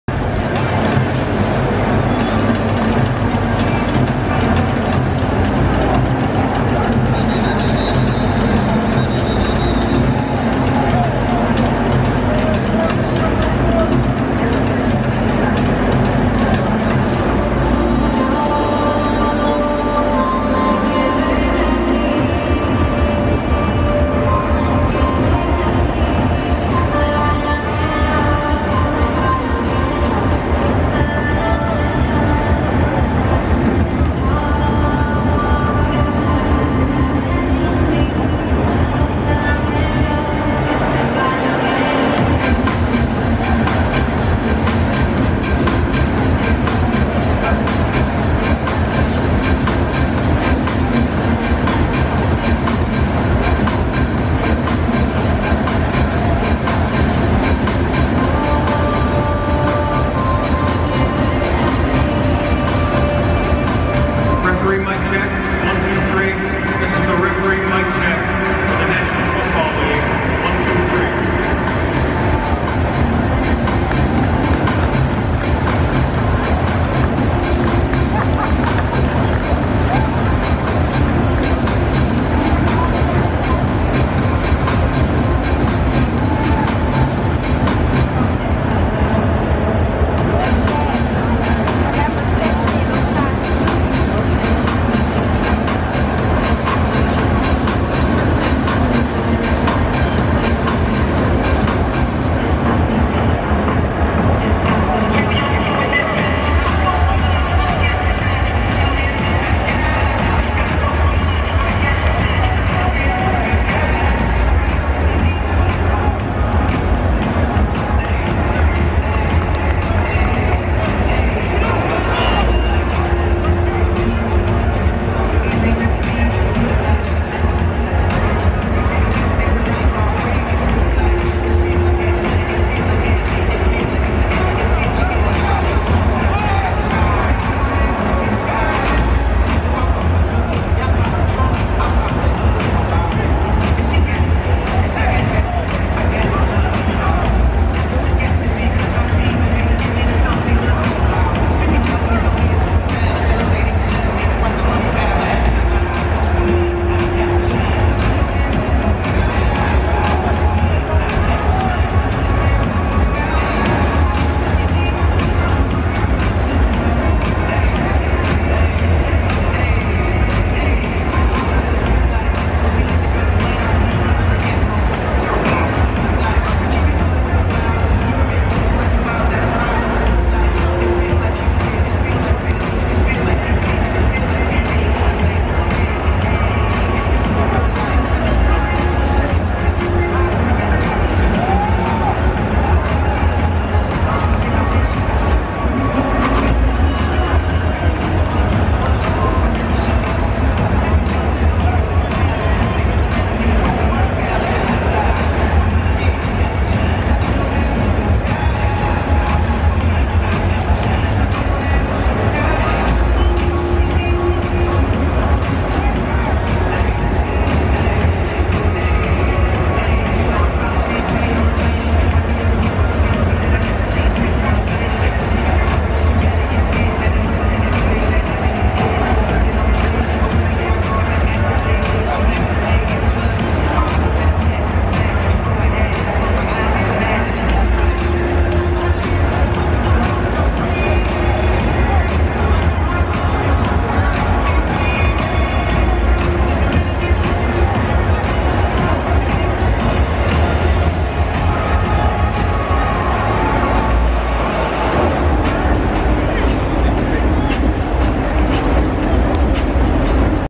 Texans vs Chargers 2022 -- Warmups #2
Texans & Chargers players warming up before their game at NRG Stadium.  October 2, 2022.